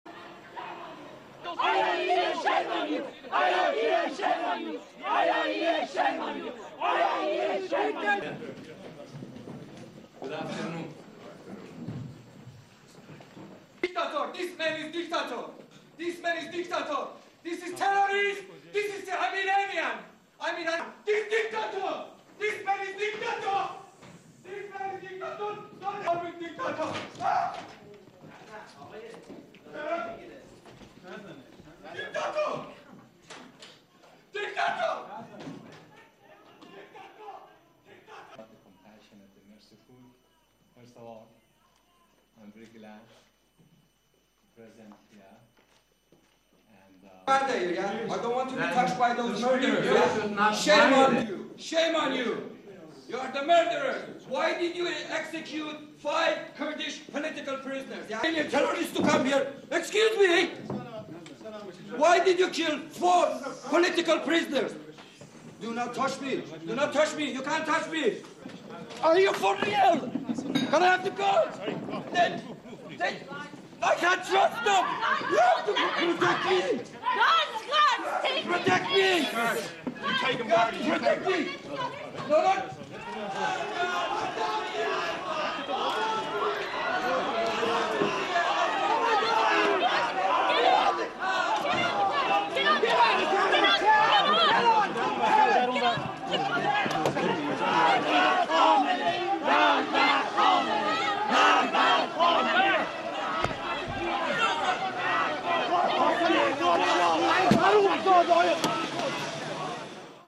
Raw Video: Men Roughed Up for Heckling Iran F.M.
Iran's Foreign Minister Manouchehr Mottaki narrowly avoided being pelted with eggs following an appearance in Dublin.  Mottaki's speech was overshadowed by angry clashes with several Iranian dissidents. (June 9)